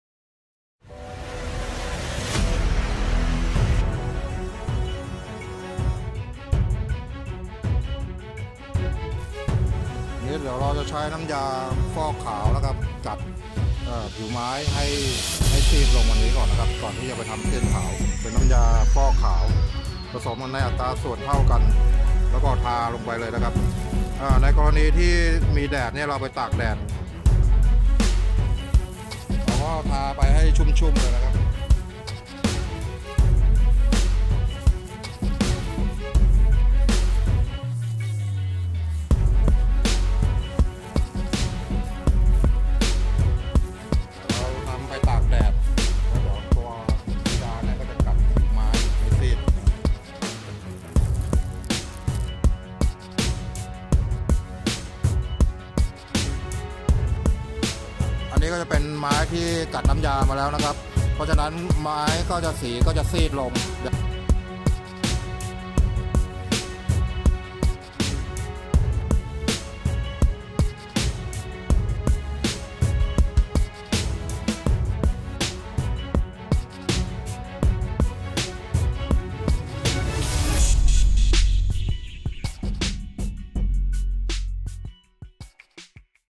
ฟอกสีไม้อัดสัก วิธีใช้นํ้ายาฟอกขาว
บางครั้งเราจําเป็นต้องฟอกไม้ให้สีอ่อนลง เพื่อนําไปย้อมสีอ่อนๆได้ หรือนําไปทําสีเสี้ยนขาวจึงนิยมนําไม้ไปฟอกขาวก่อน เมื่อนํ้ายากัดซีดลงแล้ว
เอาผ้าชุบนํ้าเช็ดคราบนํ้ายาออกให้หมดก่อนนําไปทําสีต่อไป